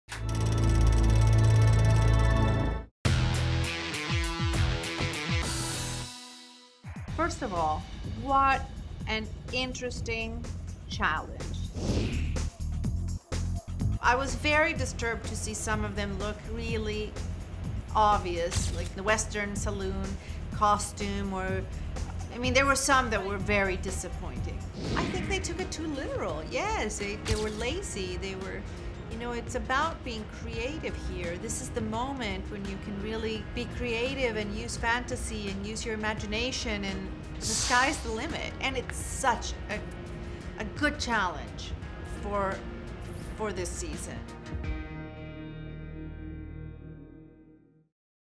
Nina Garcia's Episode 6 Recap
Nina gives her take on the movie theme design challenge.